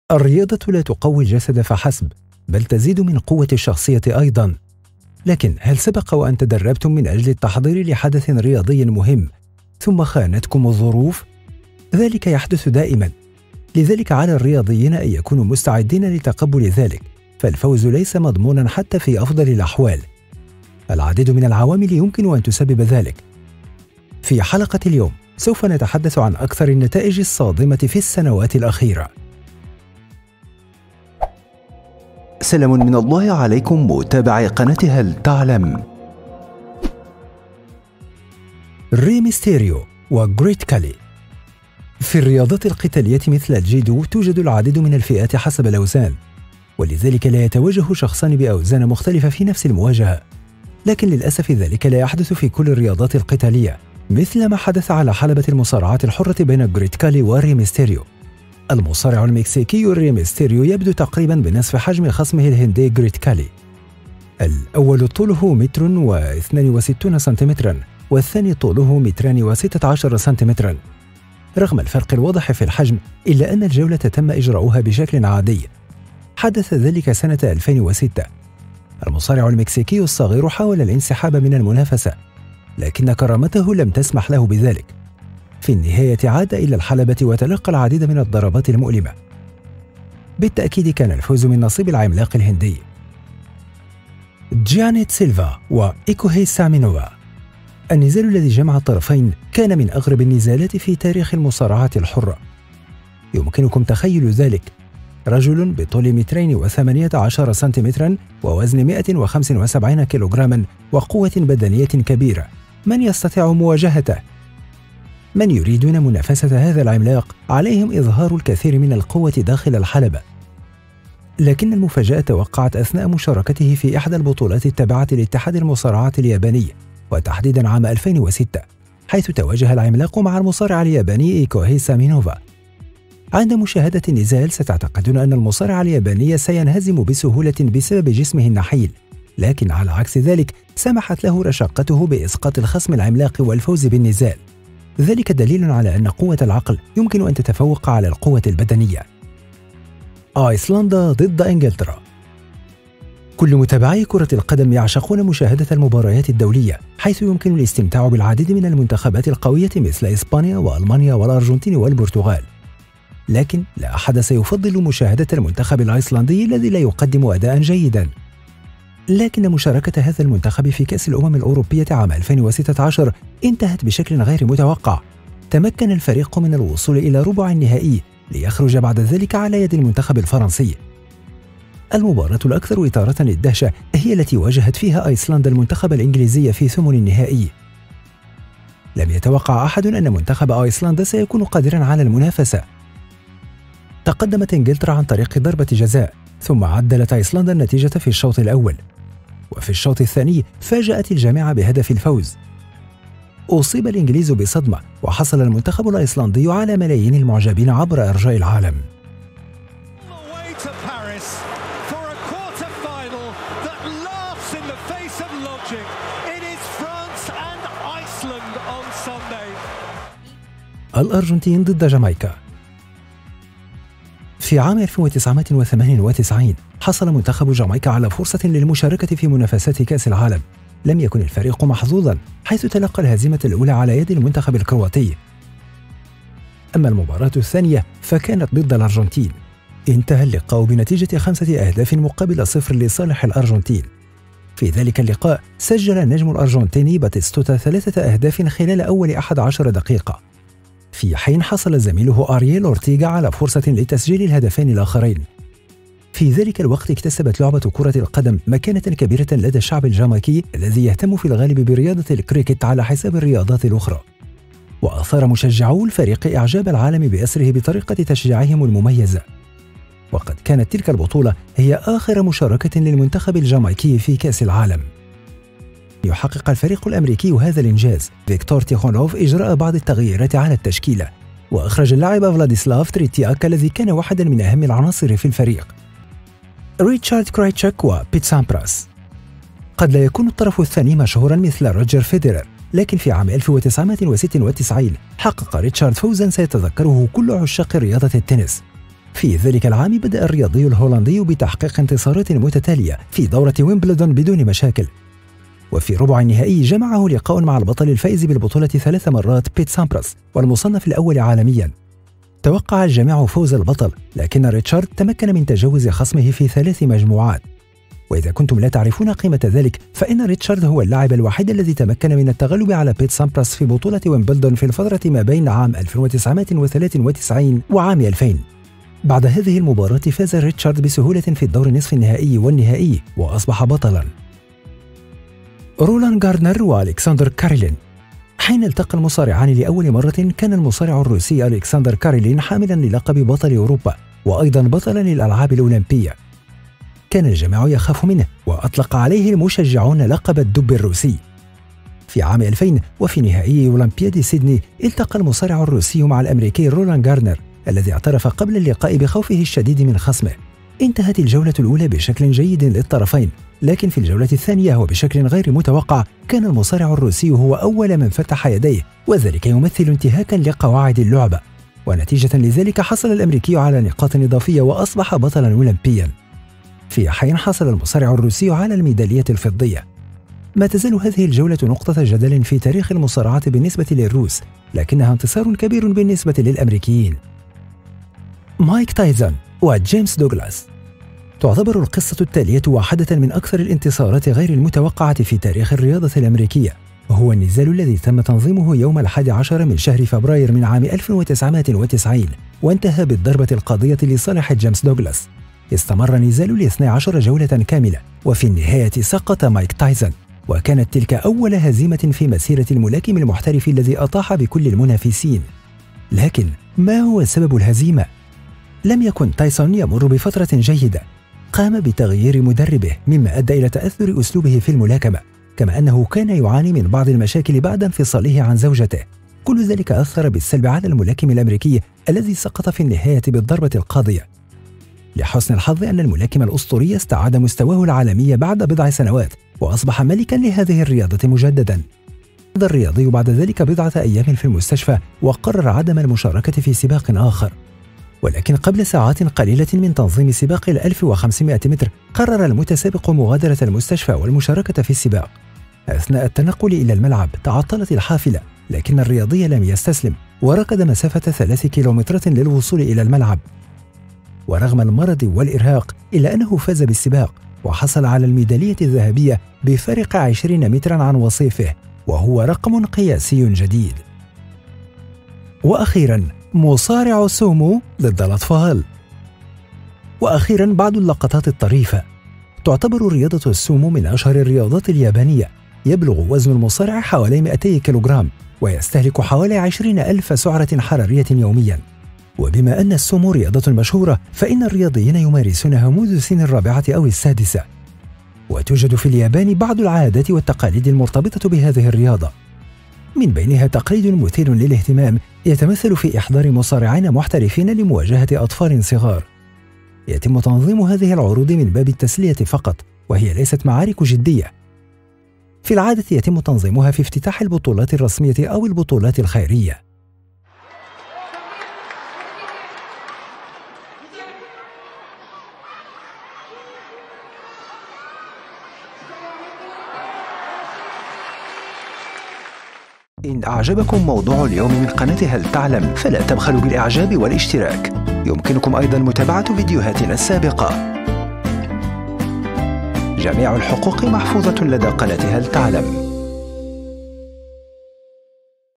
أشهر 10 انتصارات غير متوقعة في تاريخ الرياضة .. لا تصدق يجب ان تراها !!
الرياضة لا تقوي الجسد فقط بل تزيد من قوة الشخصية أيضا ، لكن هل سبق و أن تدربتم من أجل التحضير لحدث رياضي مهم ثم خانتكم الظروف ؟ ذلك يحدث دائما ، لذلك فعلى الرياضيين أن يكونوا مستعدين لتقبل ذلك ، فالفوز ليس مضمونا حتى في أفضل الأحوال ، العديد من العوامل يمكن أن تسبب ذلك و في حلقة اليوم سوف نتحدث عن أكثر النتائج الصادمة في السنوات الأخيرة.

- Google Play ‏ || التطبيق الرسمي لقناة هل تعلم؟